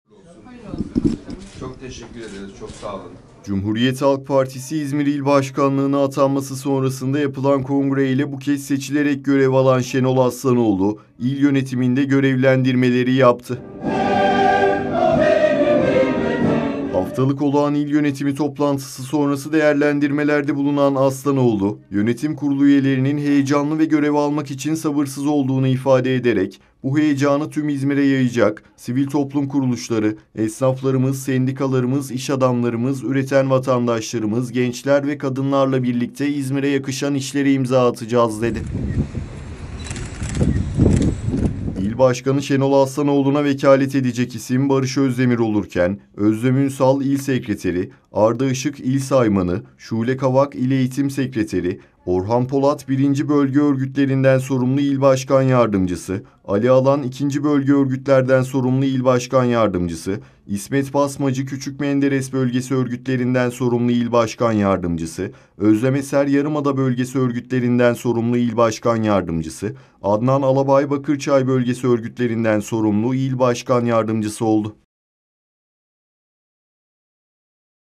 CHP İZMİR’İ YÖNETECEK KADRO BELLİ OLDU
CUMHURİYET HALK PARTİSİ İZMİR İL YÖNETİM KURULU, İL BAŞKANI ŞENOL ASLANOĞLU BAŞKANLIĞINDA HAFTALIK OLAĞAN TOPLANTISINI GERÇEKLEŞTİRDİ. TOPLANTIDA, İL YÖNETİM KURULU ÜYELERİNİN GÖREV DAĞILIMLARI DA YAPILDI.